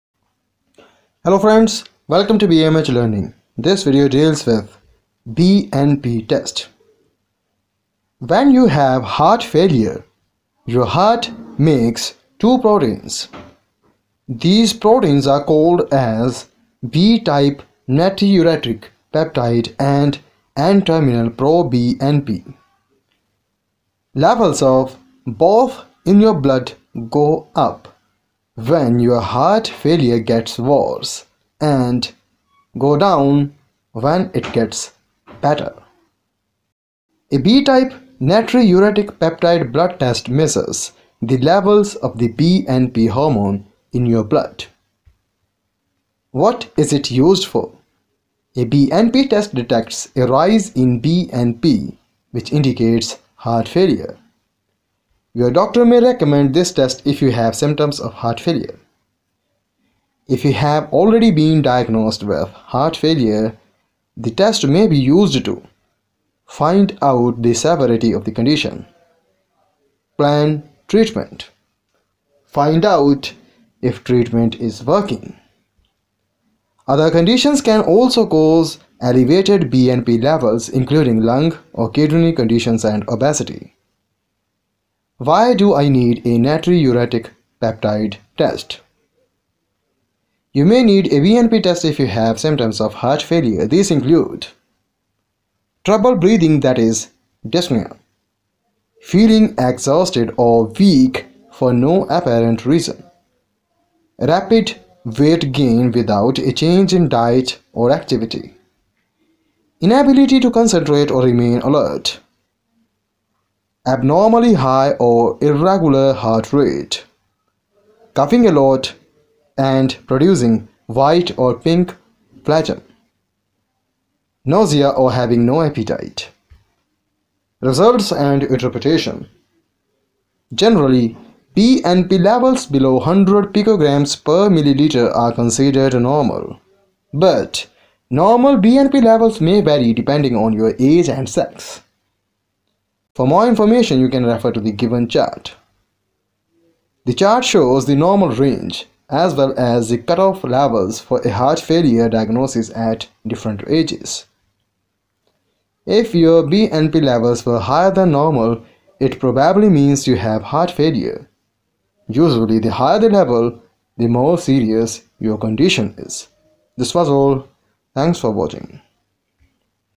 BNP Test | NtPro BNP Testing | Natriuretic Peptide Tests | B-type Natriuretic Peptide Test
When you have heart failure, your heart makes two proteins.

These proteins are called as B-type natriuretic peptide (BNP) and N-terminal-pro-BNP (NT-pro-BNP).

Levels of both in your blood go up when your heart failure gets worse and go down when it gets better.

A B-type natriuretic peptide (BNP) blood test measures the levels of the BNP hormone in your blood.

What is it used for?

A BNP test detects a rise in BNP, which indicates heart failure.

Your doctor may recommend this test if you have symptoms of heart failure

If you've already been diagnosed with heart failure, the test may be used to:

Find out the severity of the condition
Plan treatment
Find out if treatment is working

Other conditions can also cause elevated BNP levels, including lung or kidney conditions and obesity.

Why do I need a natriuretic peptide test?

You may need a BNP test if you have symptoms of heart failure. These include:

trouble breathing that is dyspnea

feeling exhausted or weak for no apparent reason

rapid weight gain without a change in diet or activity

inability to concentrate or remain alert

abnormally high or irregular heart rate

coughing a lot, and producing white or pink phlegm

nausea or having no appetite

Results & Interpretation

Generally, BNP levels below 100 picograms per milliliter (pg/mL) are considered normal.

But normal BNP levels may vary depending on your age and sex

For more information u can refer to the gievn chart

The chart shows the normal range, as well as the cutoff levels for a heart failure diagnosis at different ages.

If your BNP levels were higher than normal, it probably means you have heart failure. Usually, the higher the level, the more serious your condition is